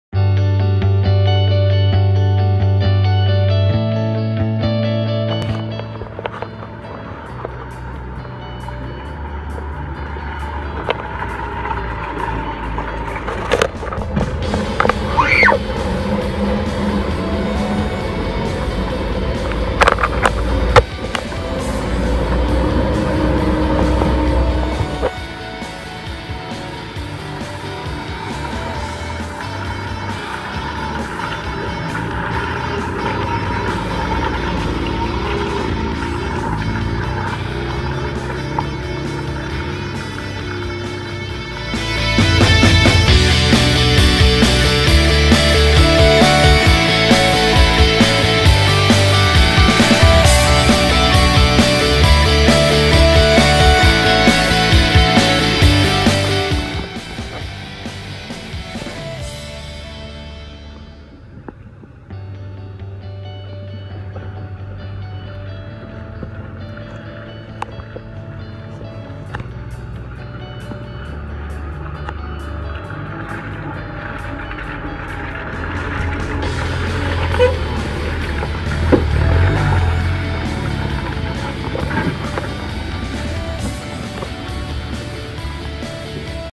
Series 2
Green laning in my Series 2 109 Land Rover in Bath